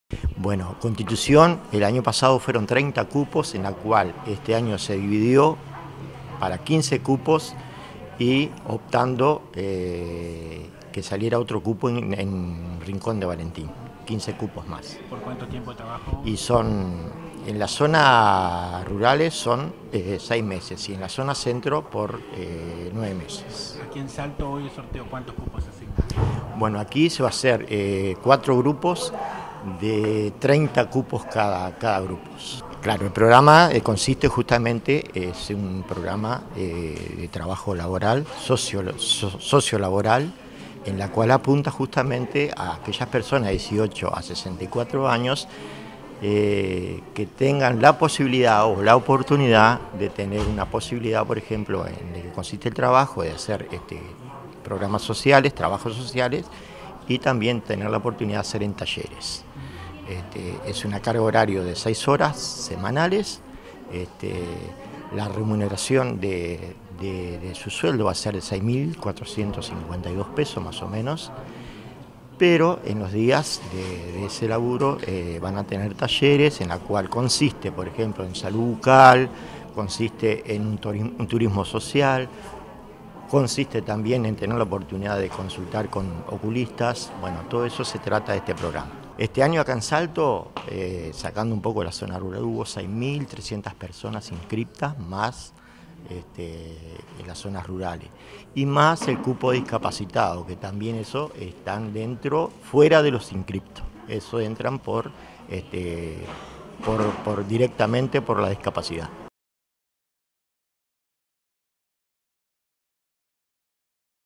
Uruguay Trabaja: se realizó el sorteo 2016
Ayer se realizó el sorteo del programa Uruguay Trabaja. Los beneficiados ocuparán 4 grupos, de 30 integrantes cada uno,  durante 6 meses. El sueldo promedio será de unos 6 mil pesos mensuales y obliga a preparación en centros educativos.